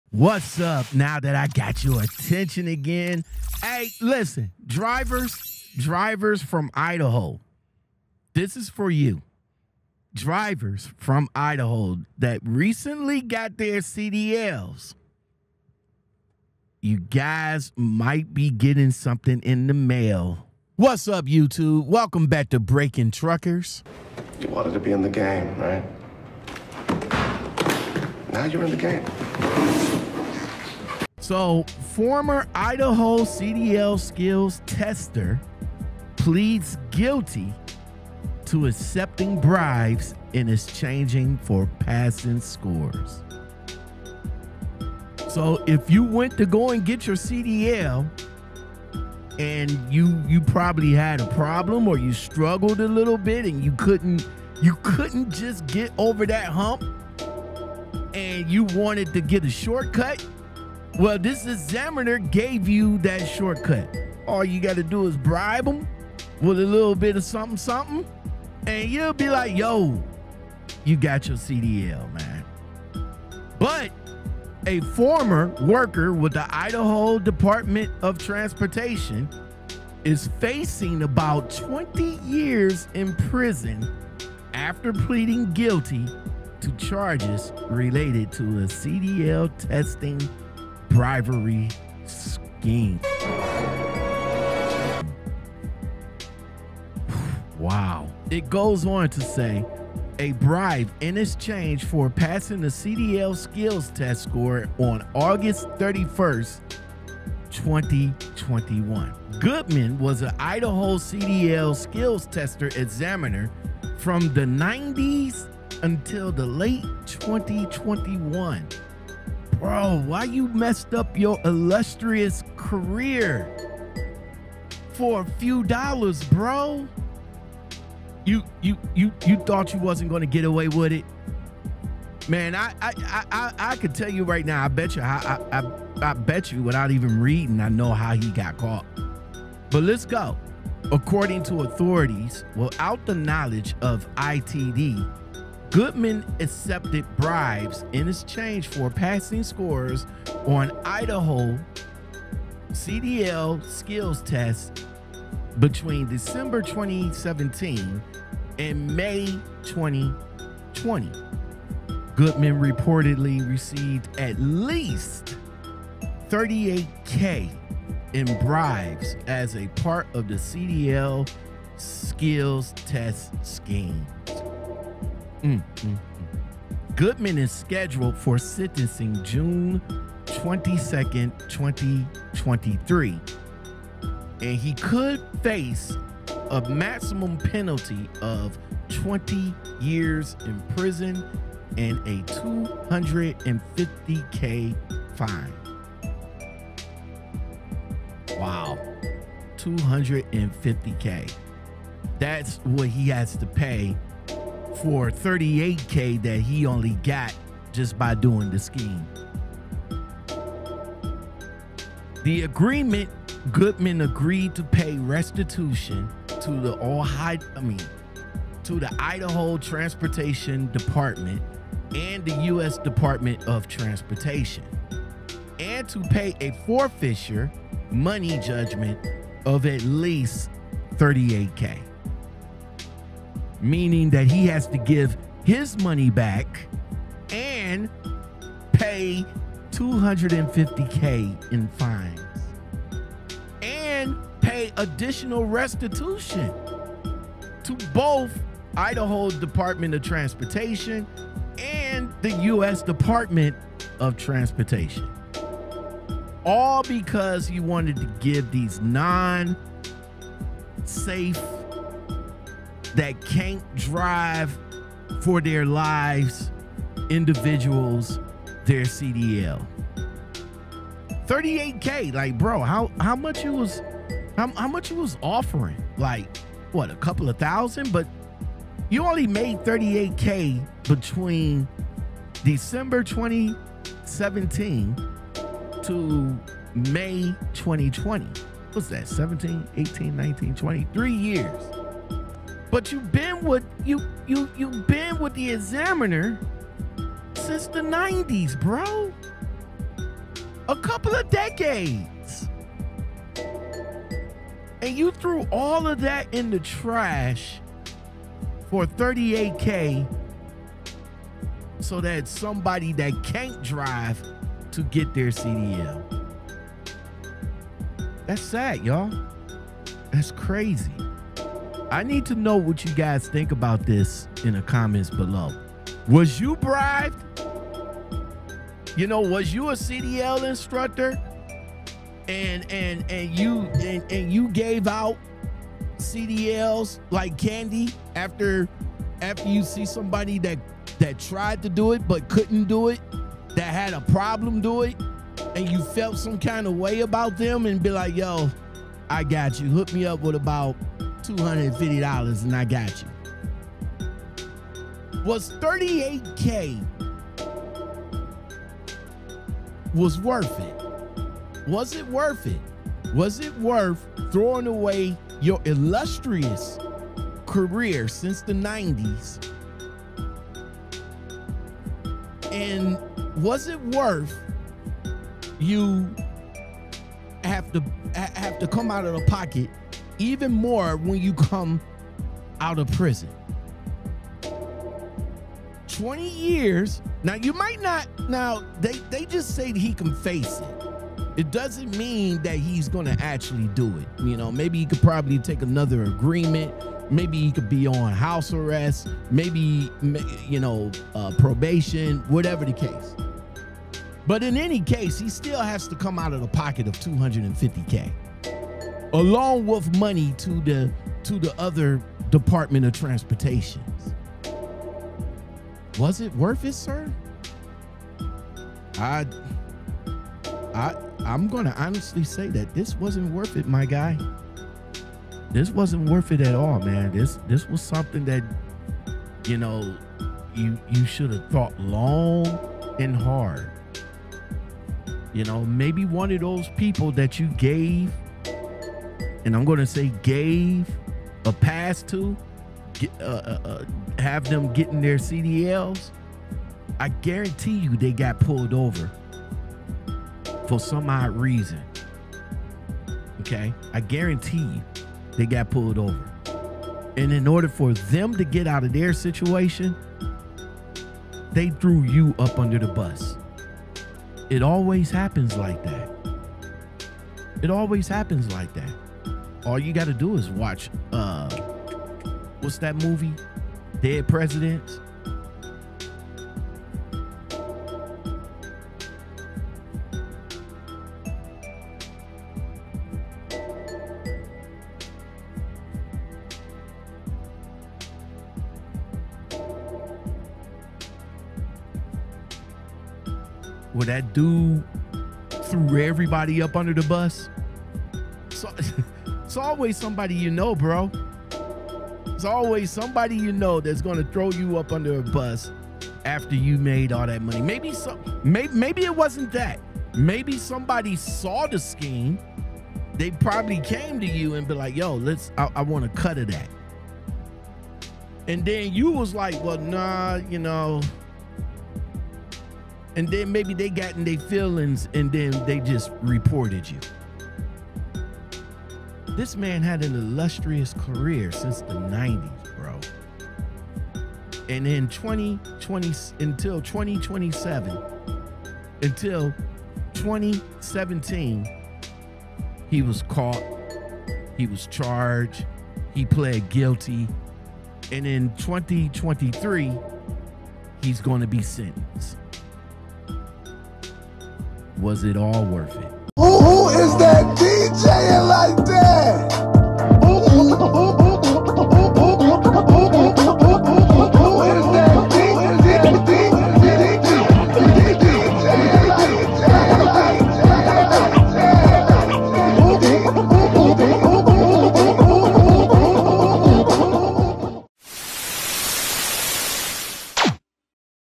FORMER IDAHO CDL TESTER TAKING BRIBESFOR PASSING TEST SCORES | The Recruiter Call Channel ☎
In Today’s Episode: FORMER IDAHO CDL TESTER TAKING BRIBES
FOR PASSING TEST SCORES

If you enjoyed this video, please...